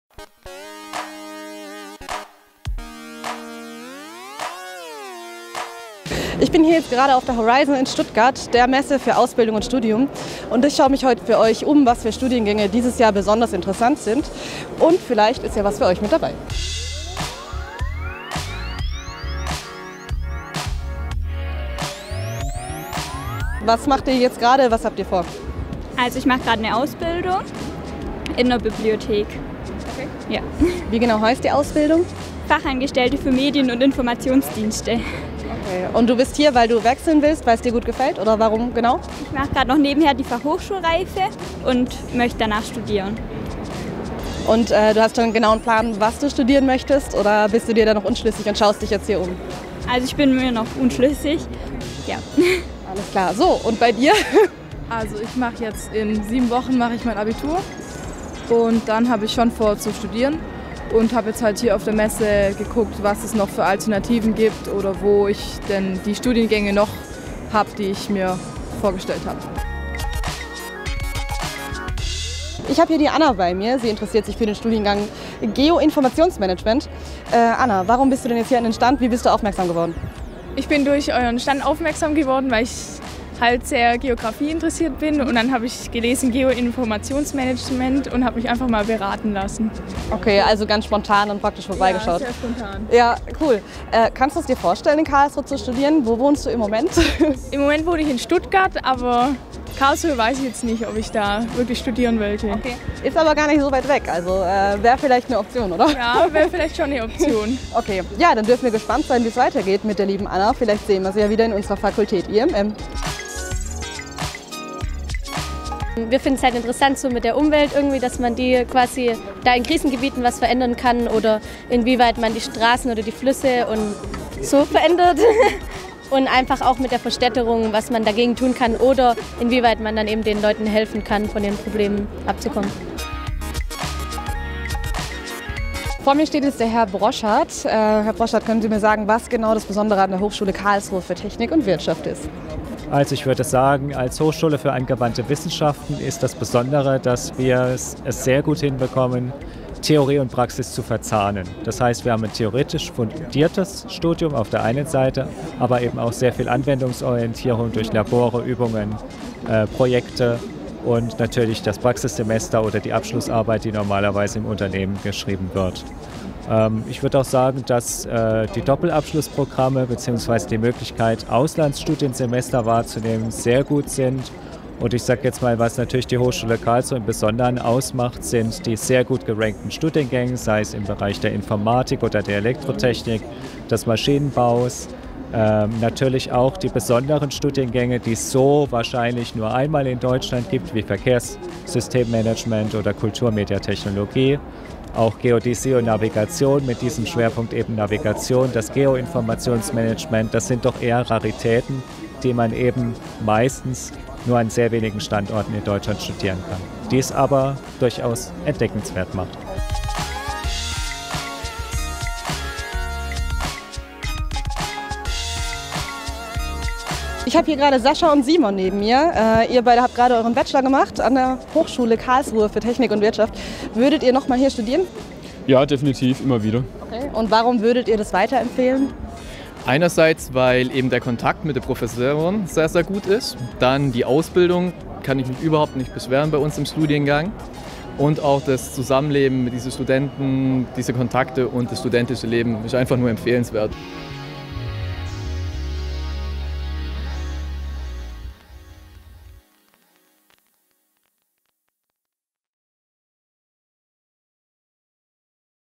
Horizon Kurzreport
Die Horizon 2015 in Stuttgart. Unsere IMM- Reporter waren live vor Ort und haben erste Impressionen eingefangen.